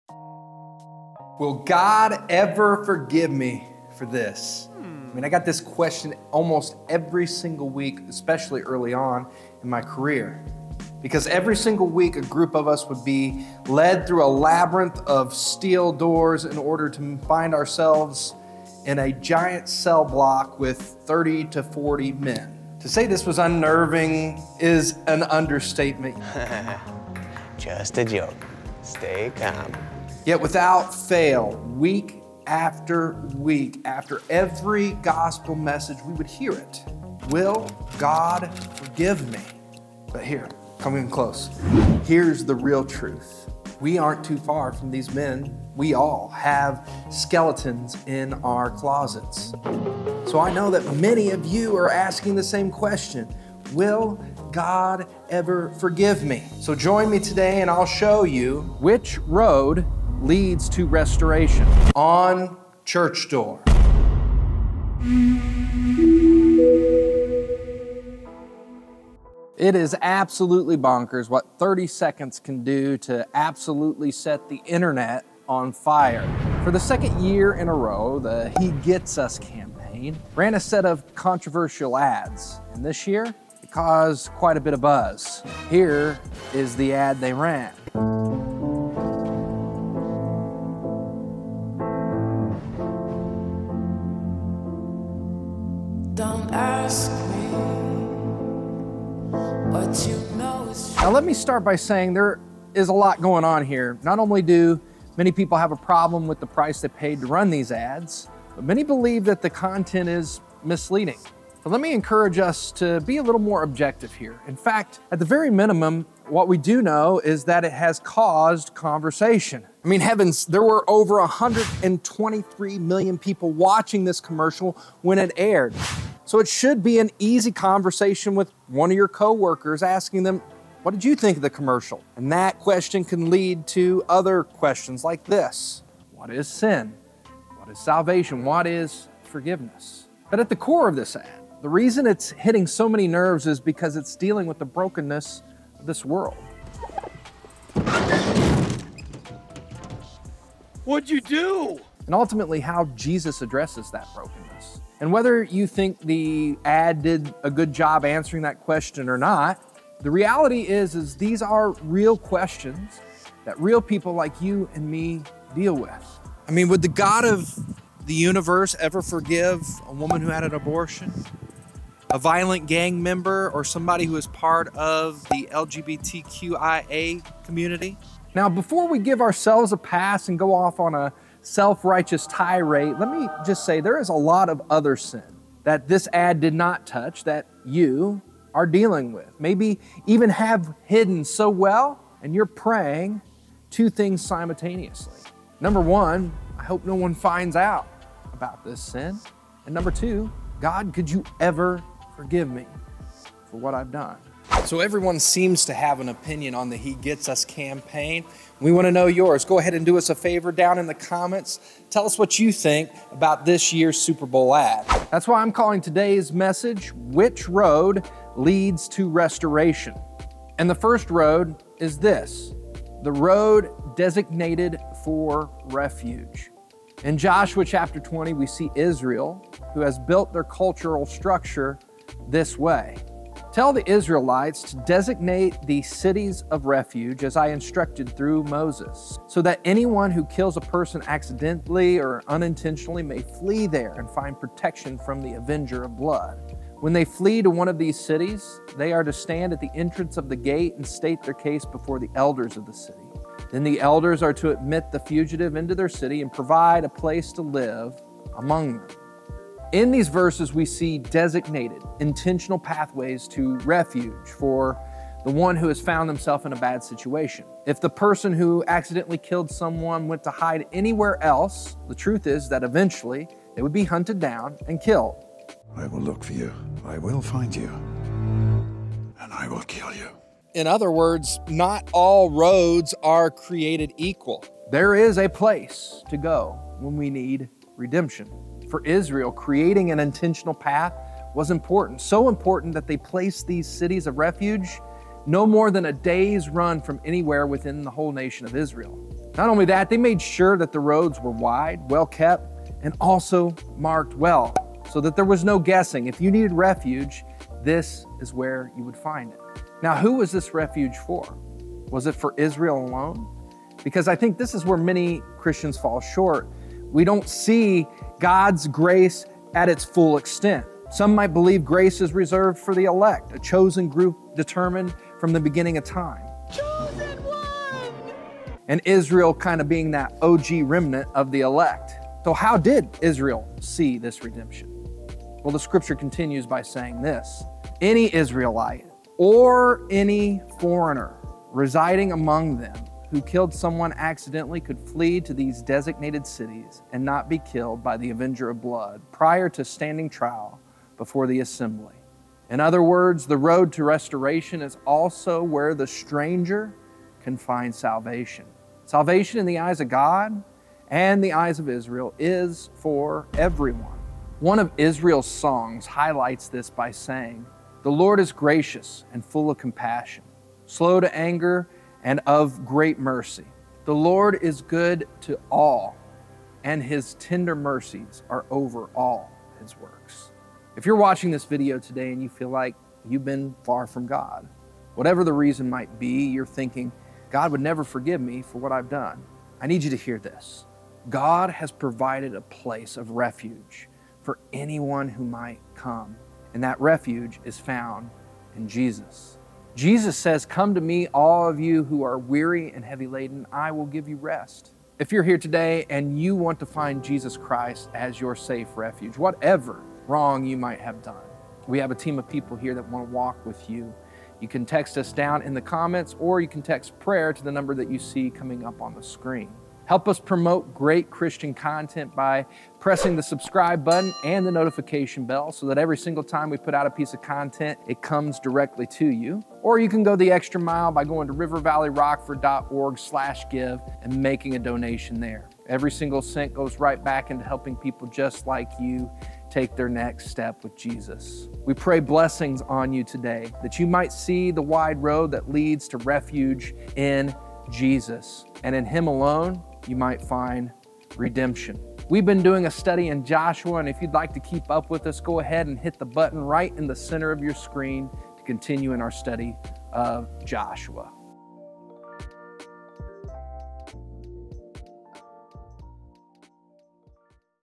Will GOD FORGIVE ME (for this...)
So many people have skeltons in the closet and they are wondering?? Will God ever forgive me for this?? They feel like what they have done is to great for him to forgive!!!

Join us today as we dig into Joshua 20 and see our God is a God that provides a place for refuge. ON Church door.